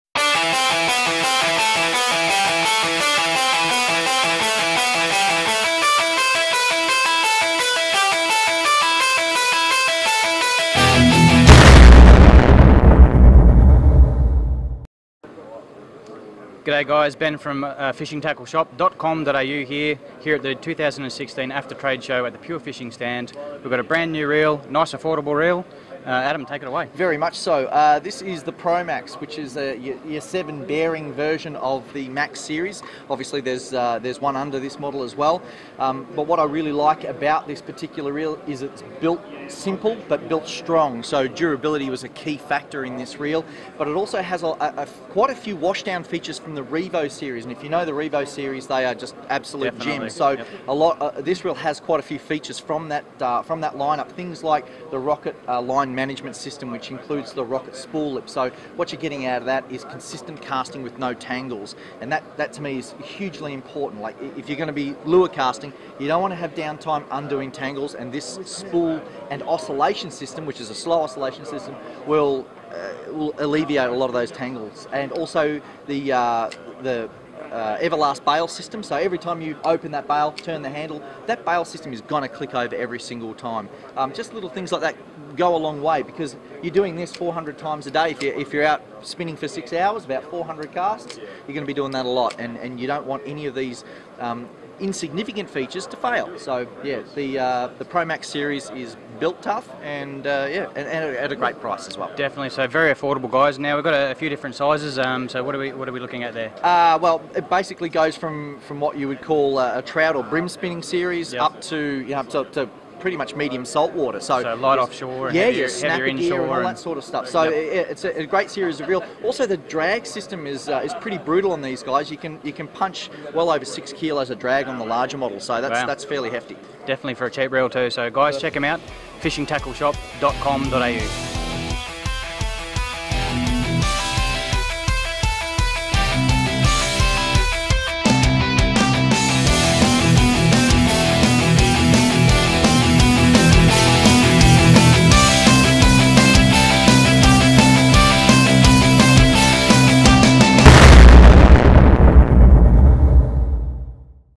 Abu Pro Max Reel - Spin Fishing Reel For Sale
Abu Garcia Pro Max Reel - Spin model fishing reel Check this reel out - Available online.